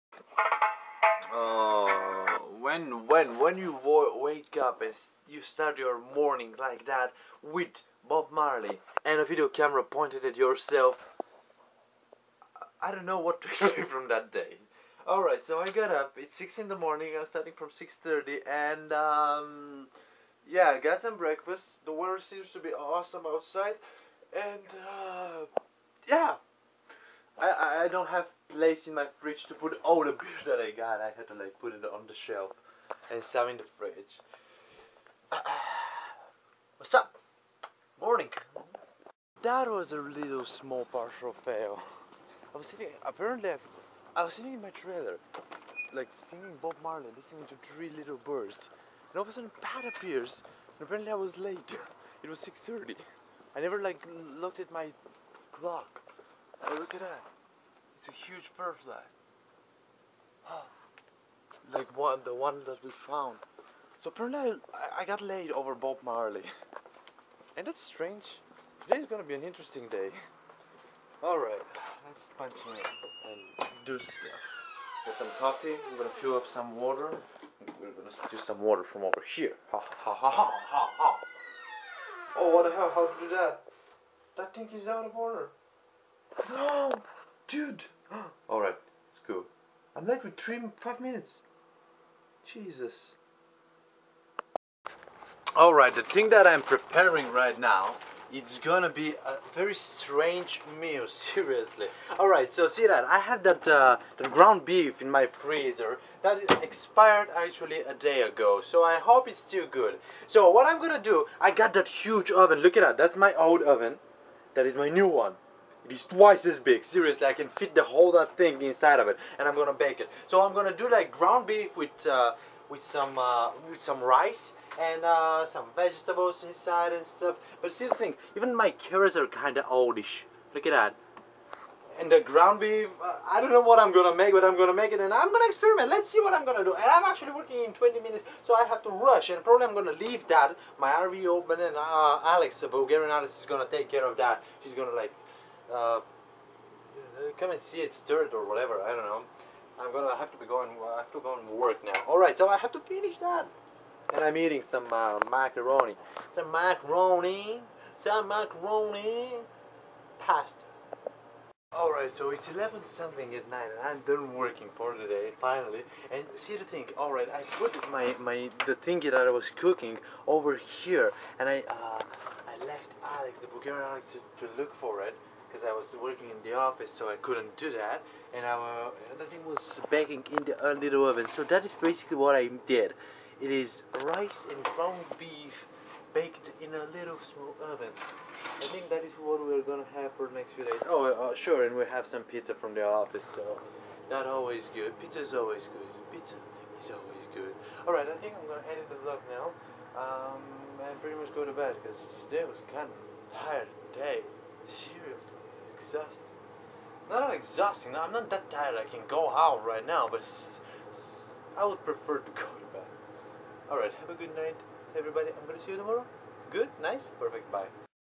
NEW OVEN MAKE NEW FOOD - 06/24/12 - 692
I seriously don't have good oven. The one that comes with the stove is just ridicules